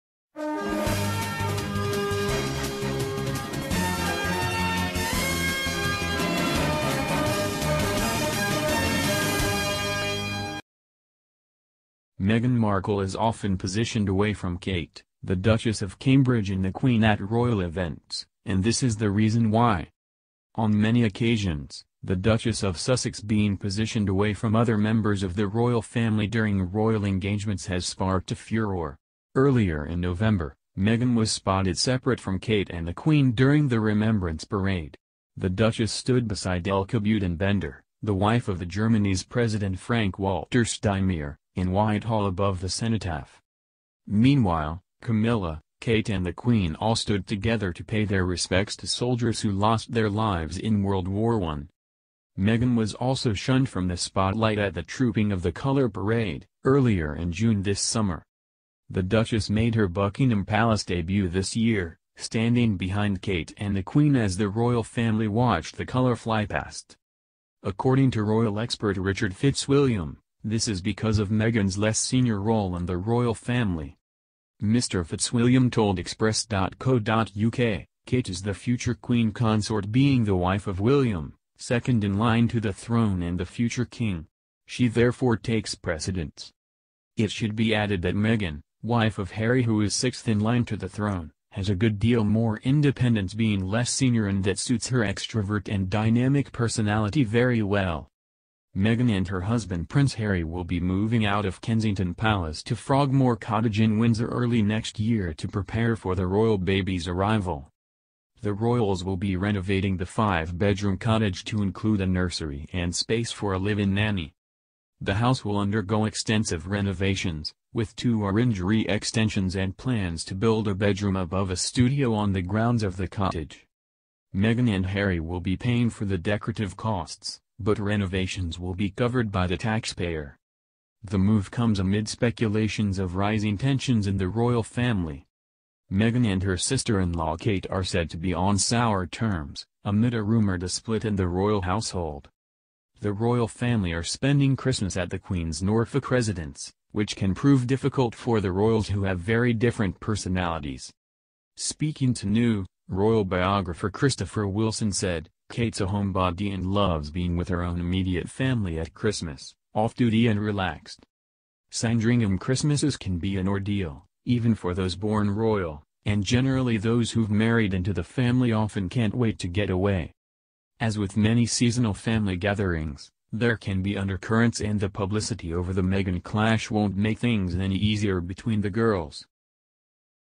Meghan felt isolated when she was alone against the criticism of Kate and the Queen
Royal SNUB: Why Meghan is OUSTED by Queen and Kate at royal events - reason REVEALED